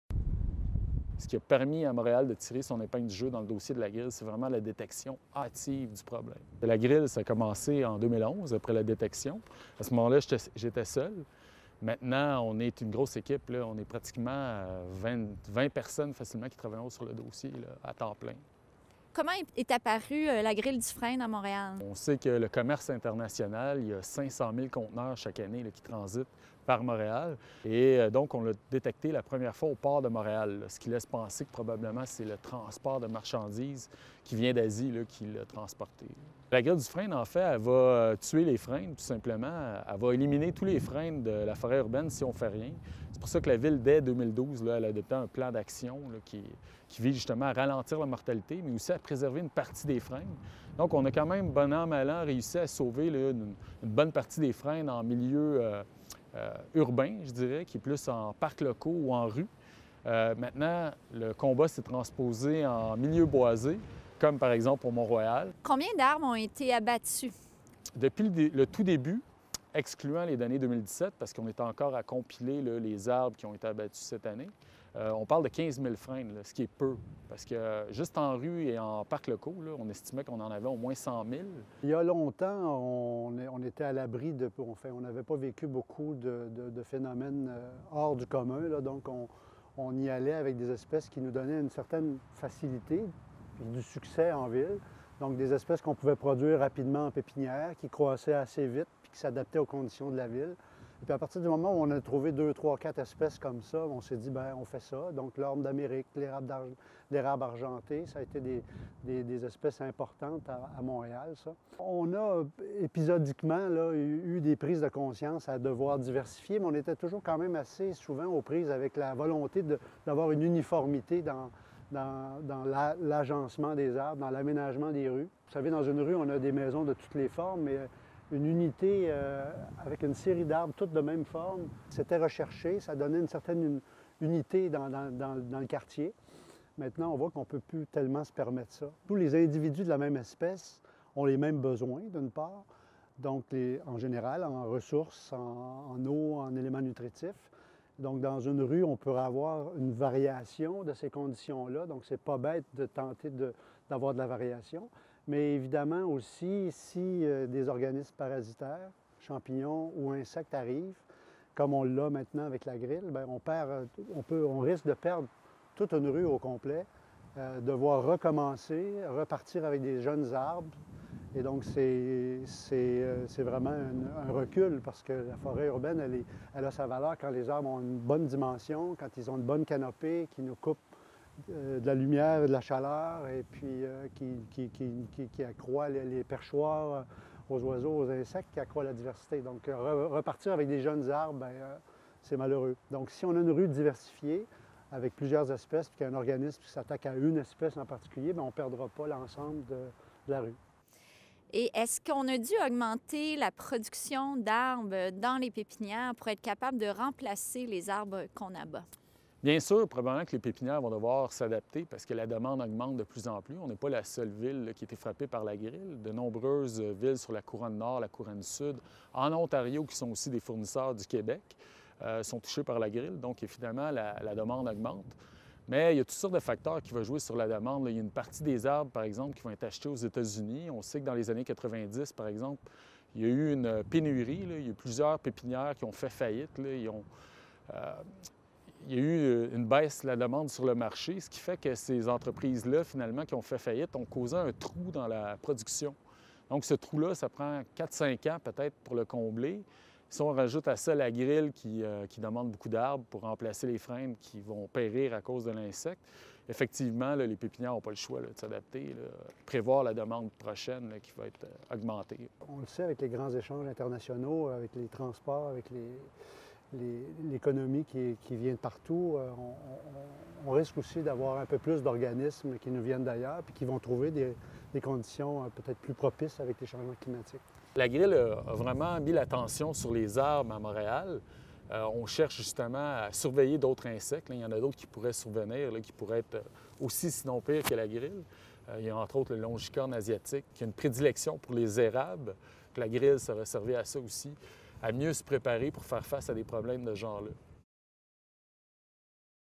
L’importance de la biodiversité
L’agrile du frêne a attiré l’attention sur les arbres à Montréal. La forêt urbaine est-elle bien protégée contre d’autres insectes ou d’autres organismes parasitaires? Pour écouter le débat qui a suivi mon reportage, rendez-vous sur Mise à Jour Montréal